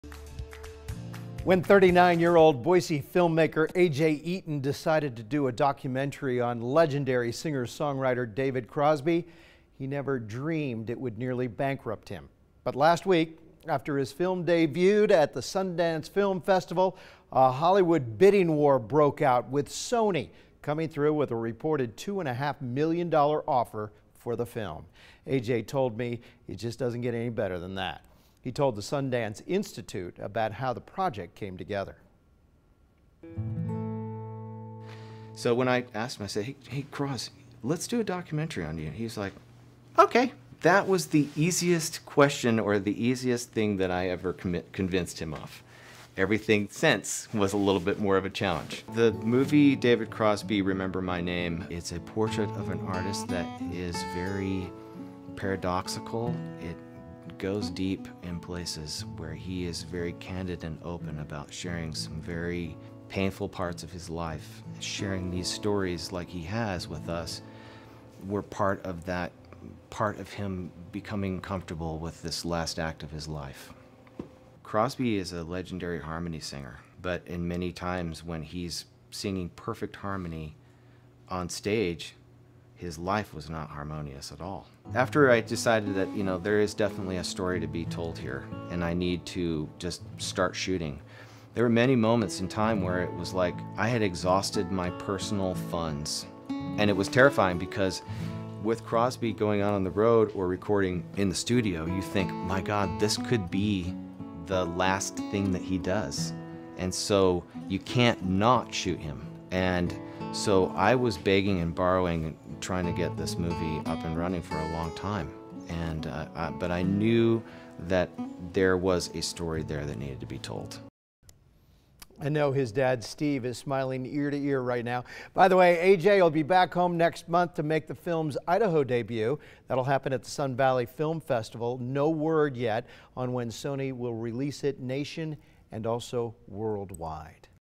Blockbuster hit for Boise filmmaker
A bidding war for the film broke out after it debuted at the Sundance Film Festival.